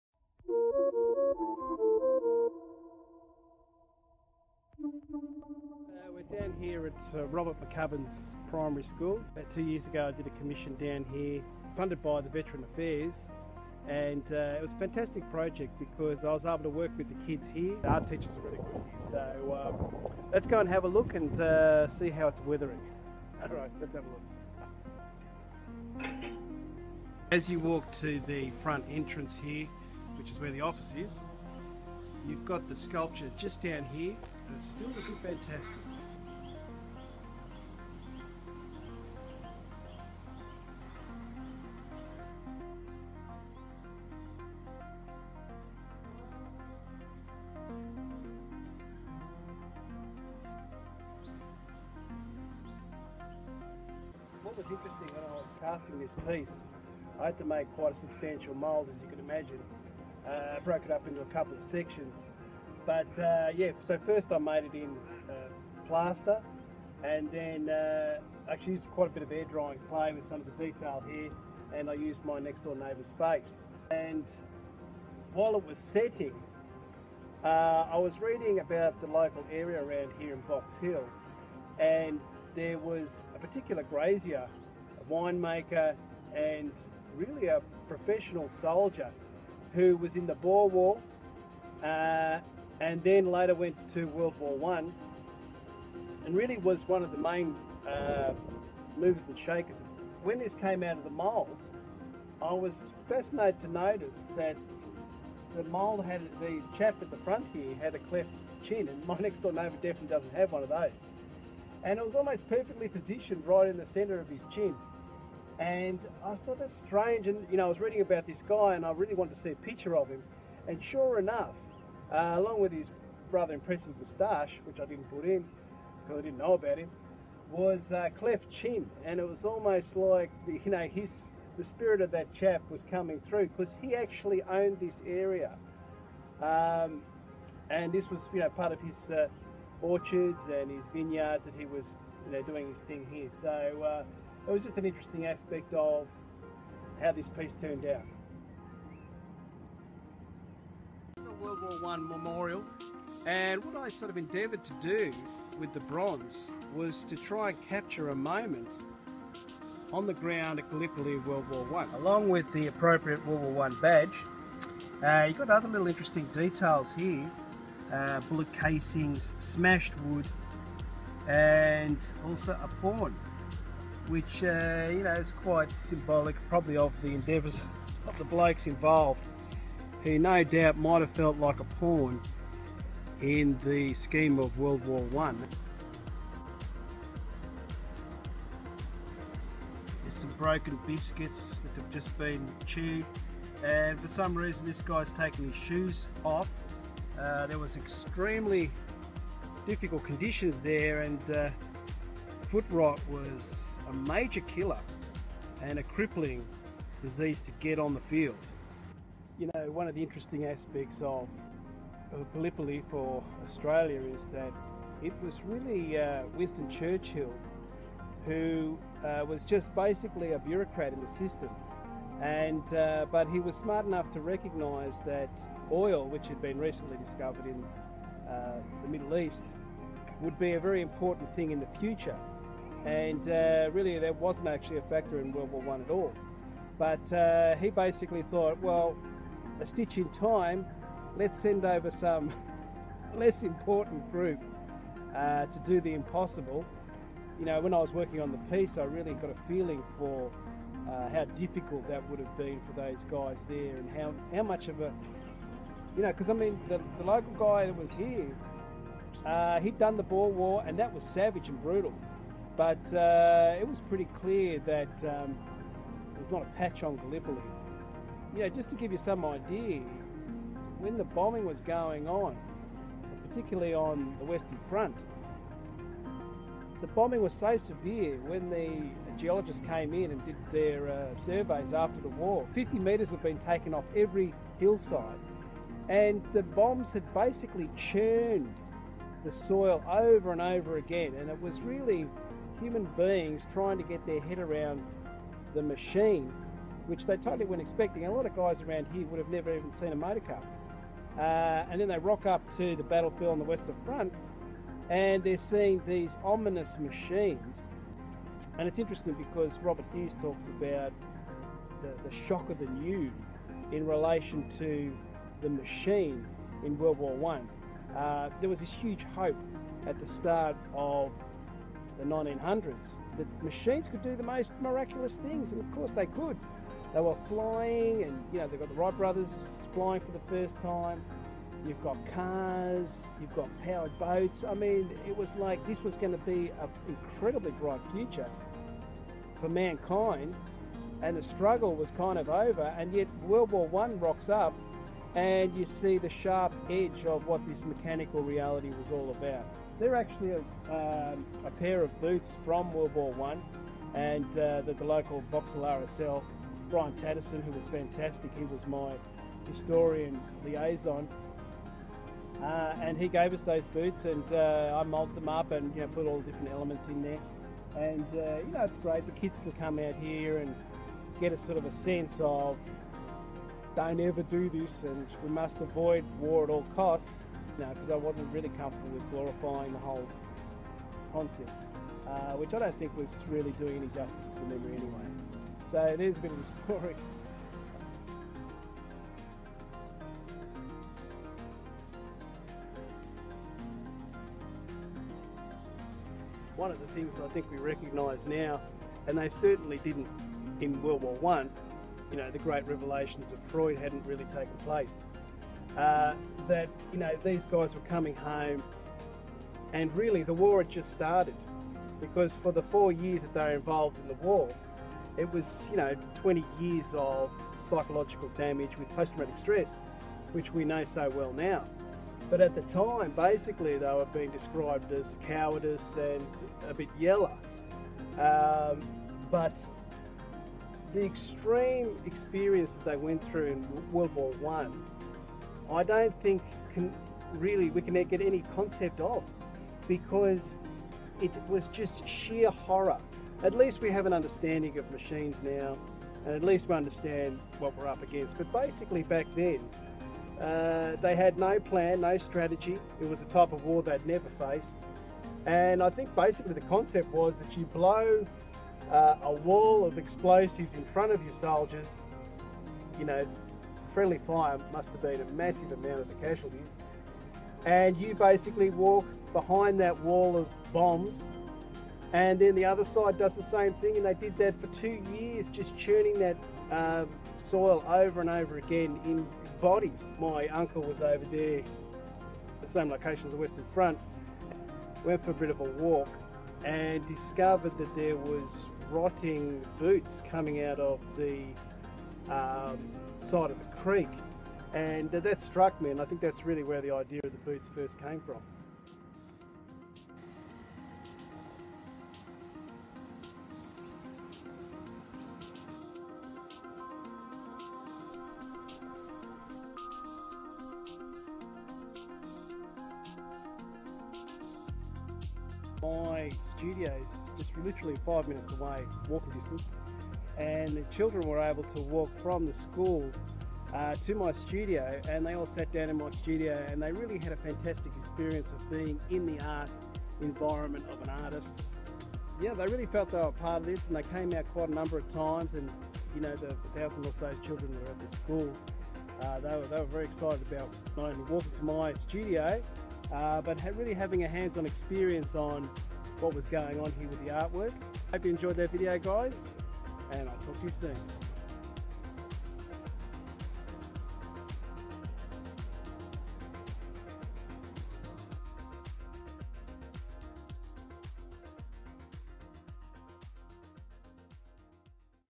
Robert Mc Cubbins PS Adrian Ward
Somme Survivors WW1 Mural by artist Adrian Ward.

In commemoration of the 100 years since WW1 sculptor, Adrian Ward has created an impressive mural that captures a moment in time, at the Somme Battlefield in France.

Soldiers returning from the front line walk a well-trodden path along the trenchers to possibly safety but defiantly the unknown.

“I used actual photos from the trenches,” says artist Adrian Ward, thankfully supplied by Brian Tatson from the Box Hill RSL memorial department. “One of the photos was from a captured German trench which I then used in the mural. From the photo, I incorporate the heavy wood tank bridge, positioned on top of an ailed tank from the time.”

Adrian explains how the soldiers must have felt seeing a tank for the first time. “Mechanical machines had only been around for 16 years or so, the soldiers must have been shocked to see the industrial destruction they had volunteered for.”

“Under such pressure, the soldiers must have had to rely on each other to survive physically and physiologically. I have endeavored to portray this in my portrait of this time in history which was the start of the age of machines, ” says Adrian.

The Soldiers in the mural are trekking their way through the trenches to the possibility of safety, from the distance to the front at the center of the mural. The artist has created this effect using a scale perspective where the figure starts at 100mm high and finish with the central character, which is life-size.

With the life-size central character, the artist has used a real army jacket which he transformed into cement. “Transforming items of clothing into cement is also a part of an exhibition of my artwork that I am working towards. Most of the other artworks in the exhibition are cast in gold and silver bullion. ” says the artist.

While the artist Adrian Ward was busy creating the artwork, the grade 6 students from Robbert Mc Cuban Primary School made regular visits to Adrian's studio, just 10 minutes’ walk from the school. Students were engaged in watching the development of the ideas, the artwork and the final product which the student added to with a handmade frame of ceramic poppies tile for the mural. The first version of the mural with the poppy tile can be seen in the War Memorial Garden at Robbert Mc Cubbin Primary School.
Interest in the mural has gathered momentum, opening the possibility of the artwork to be cast in bronze.

“Over the last 5 months creating the mural, I felt I was going back in time every time I went into my studio to work. If I have been able to transport people’s minds back in time, I hope I have succeeded in communicating the spirit of mateship the digger had for each other and the battle that was just beginning for a soldier returning home.” Said artist Adrian Ward.